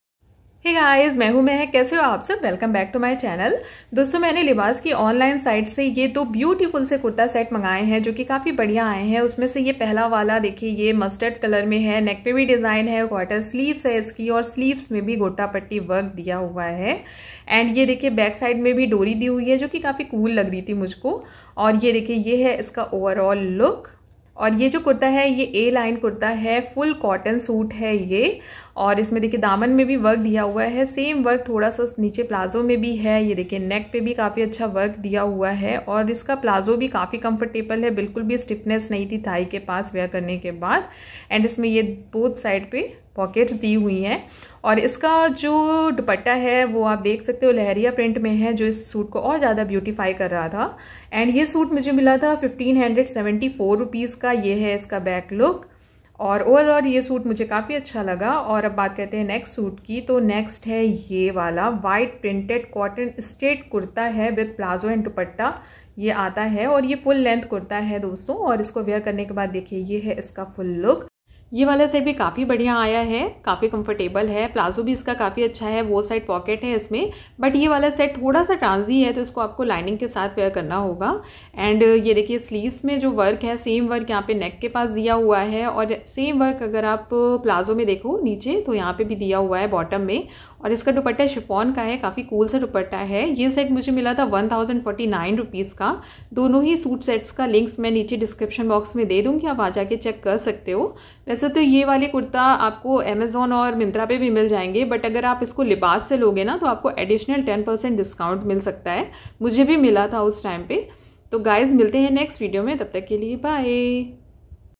Affordable Libas Kurta Sets Haul
Affordable Libas Kurta Sets Haul | Kurta Sets

Purchase Link
Libas Online Site
Checkout this product Mustard Yoke Design Cotton A-Line Kurta With Palazzos & Dupatta

Amazon Links
Libas White Printed Cotton Straight Kurta With Palazzos & Dupatta

Girl or Women A-Line Kurta With Palazzos Mustard

Boya M1 Mic
Ring light
Green Screen Kapda
Camera Tripod
Smart LED Bulb